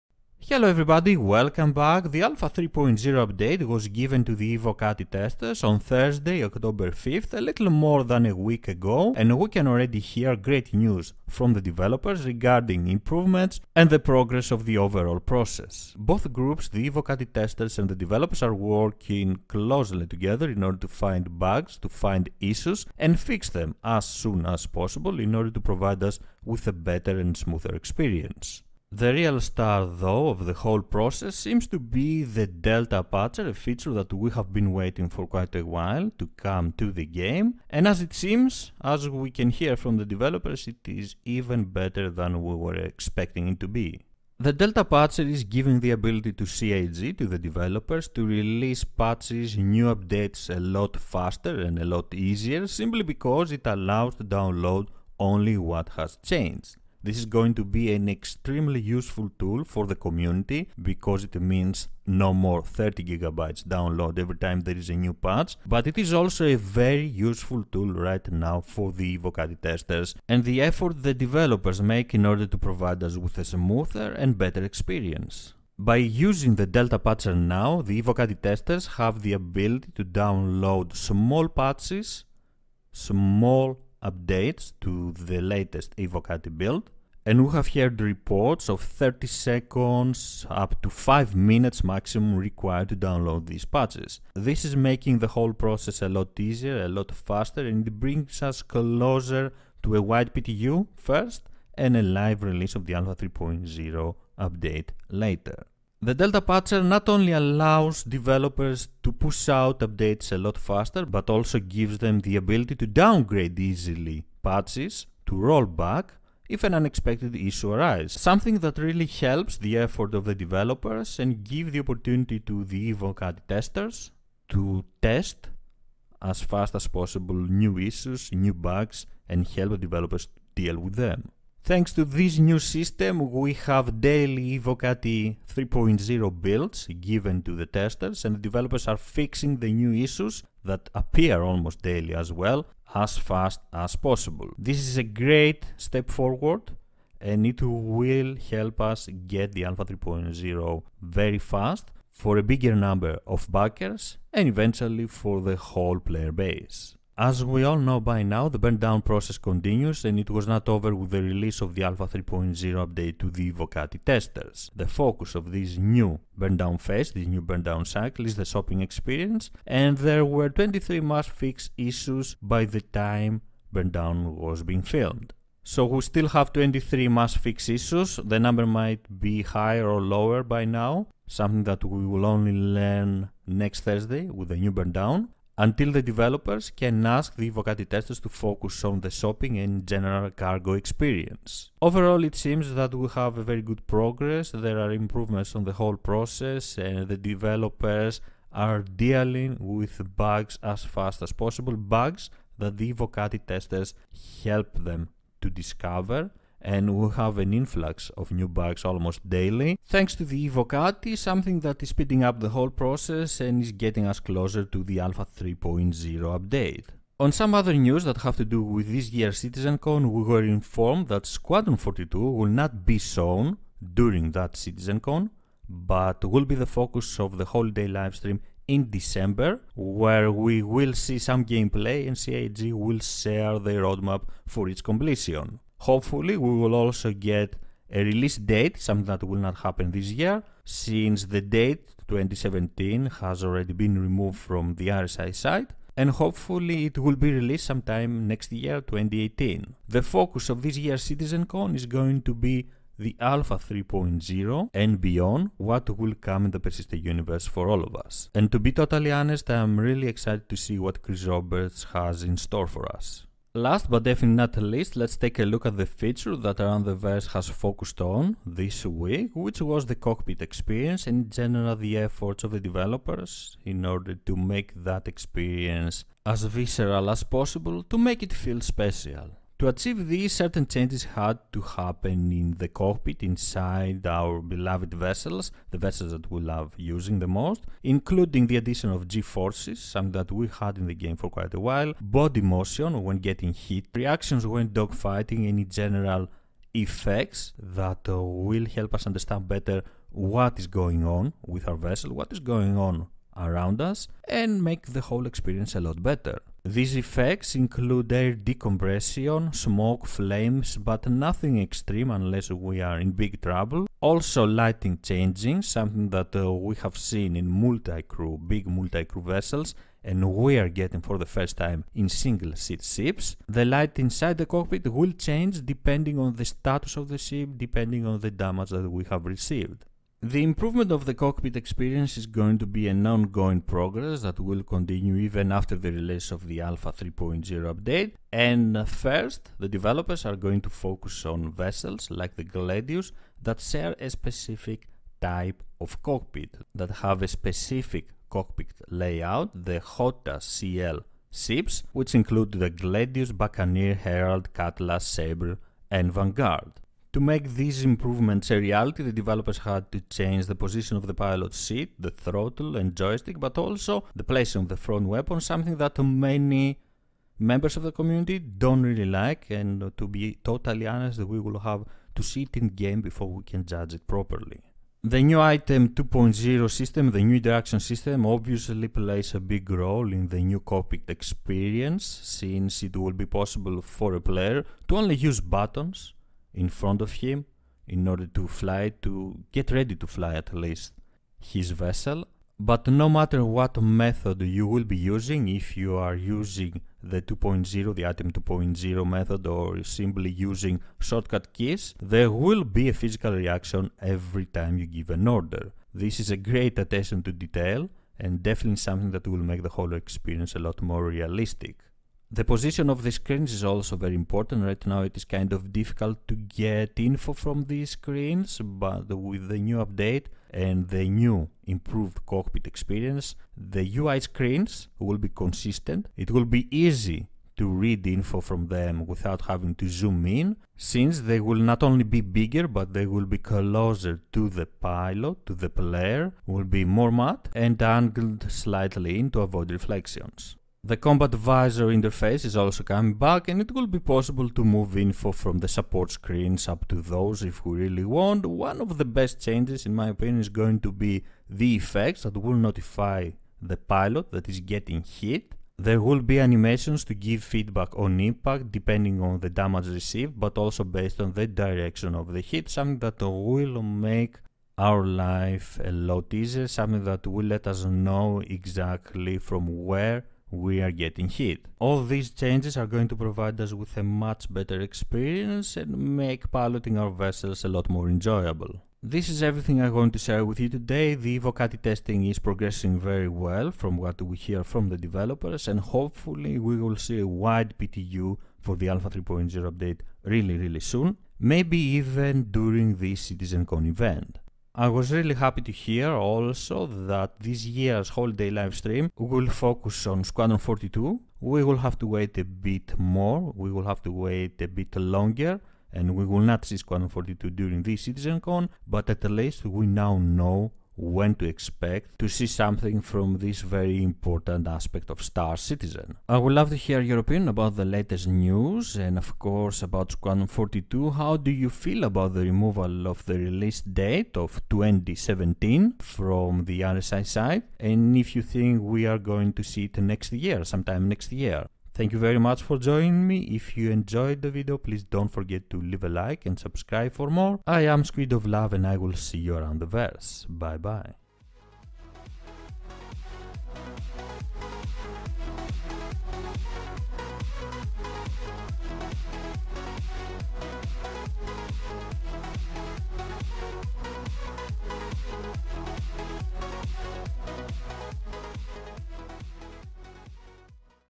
Star Citizen News | Citizencon 2017 - Delta Patcher
Let's take a look at the latest Star Citizen News. Taking a look at the latest news regarding citizencon 2017 but also the delta patcher.

and gain 5000 UEC !

0:28 delta patcher
2:37 burndown
3:37 citizencon 2017 news
4:23 cockpit experience